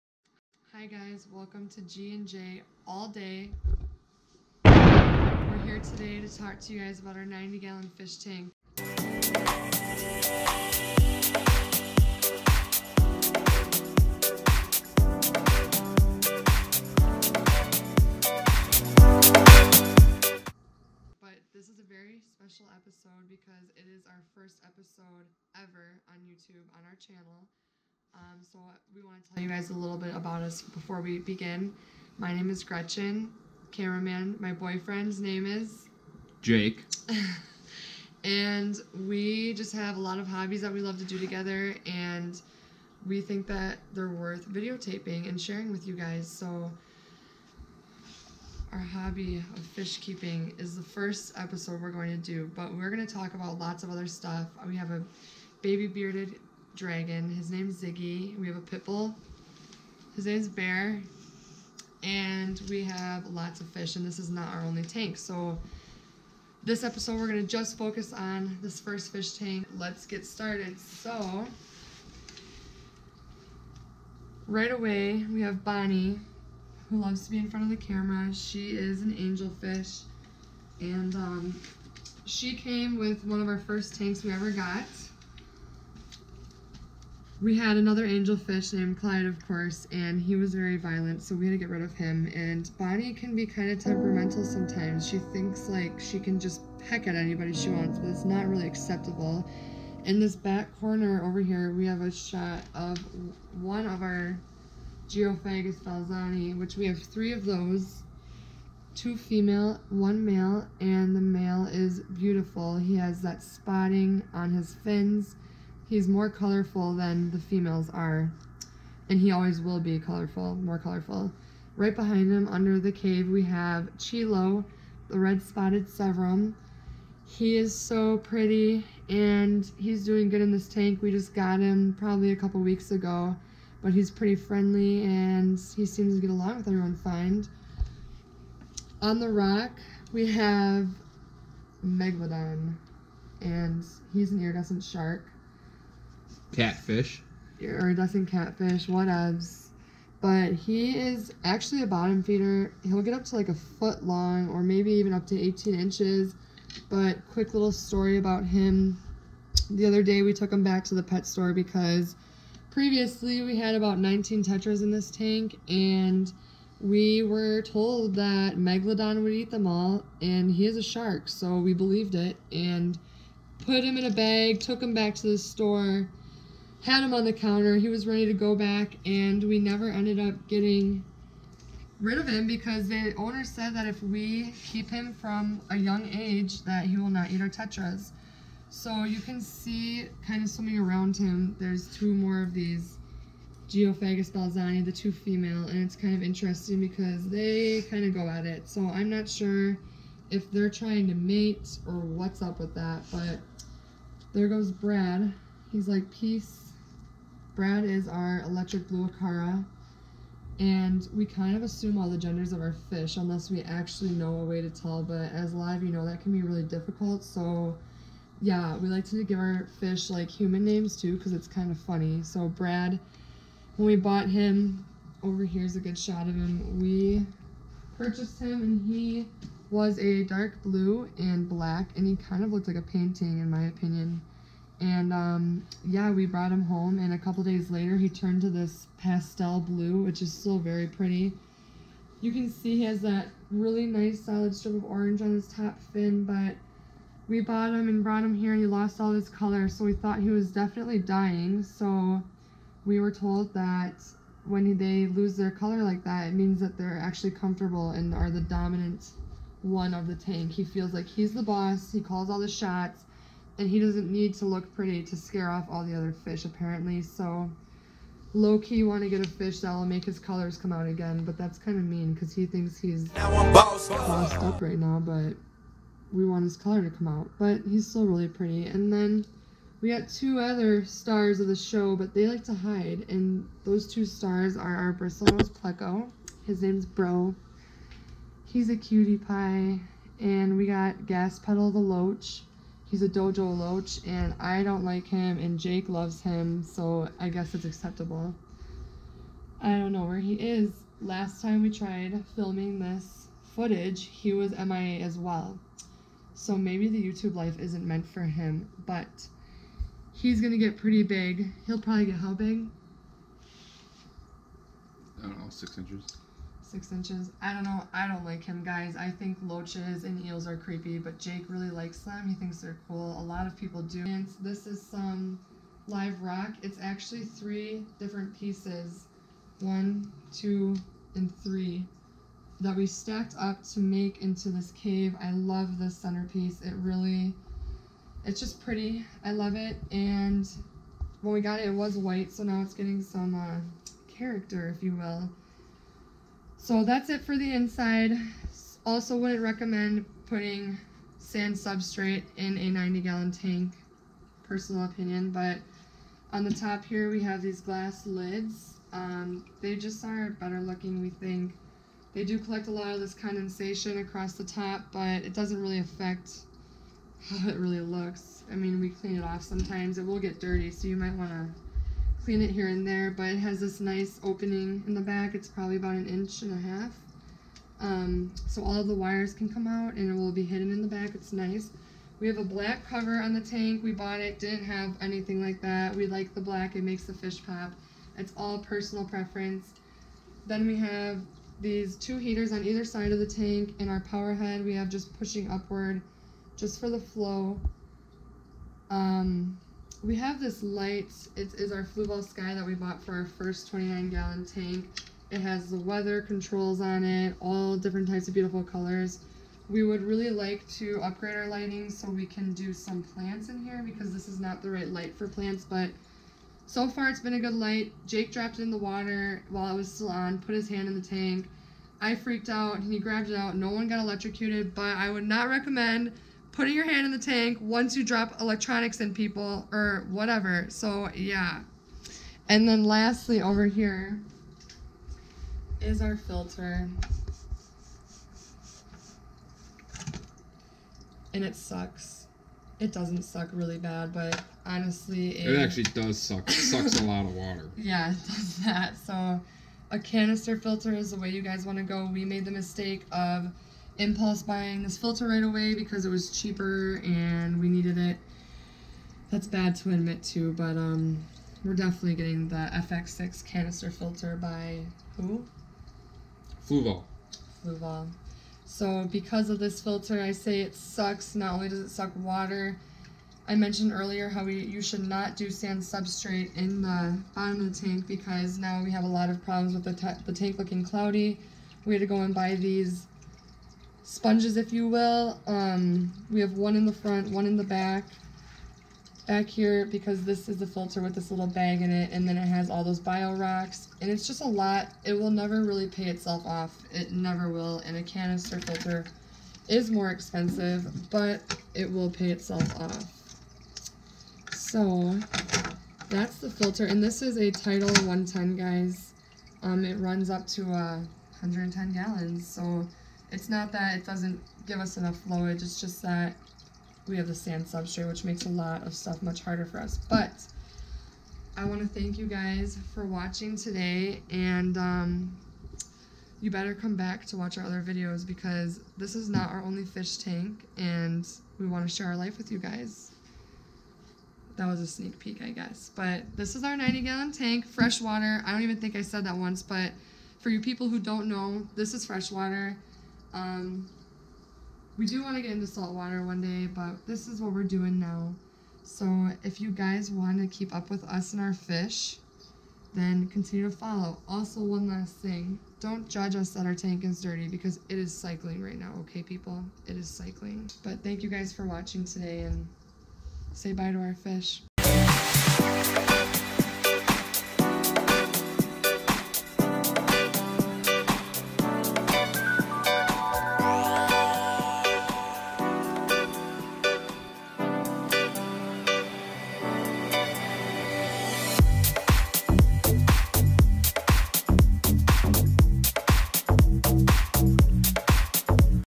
90 GALLON FISH TANK!!!!!!!!!
Our 90 GALLON TANK! First episode EVER. Discussing our fish, their tank, filters & lights.